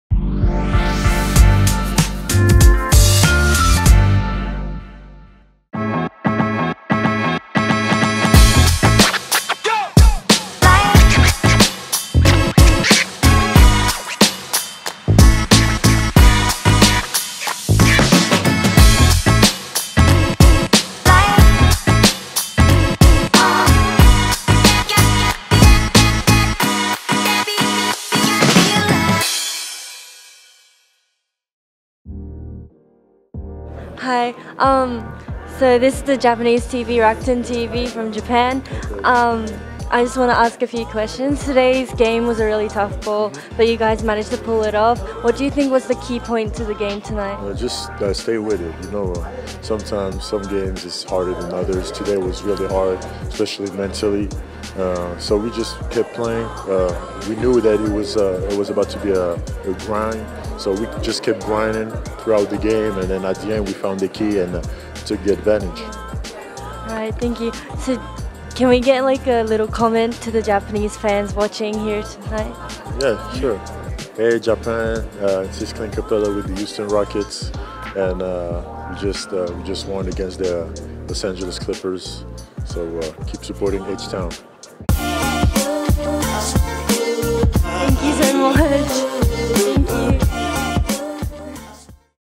Rockets - クリント・カペラ - Clint Capela - 選手にインタビュー！【Rakuten NBA 32】
Rakuten NBA 32は各チームにアンバサダーを任命！
現地での活動を「Rakuten NBA 32 公式YouTubeチャンネル」にアップしていきます！

今回の動画では抜群の身体能力を誇るロケッツの若手プレーヤー「クリント・カペラ」選手にインタビューしました！

We are Rakuten NBA 32, have assigned the ambassadors on each team.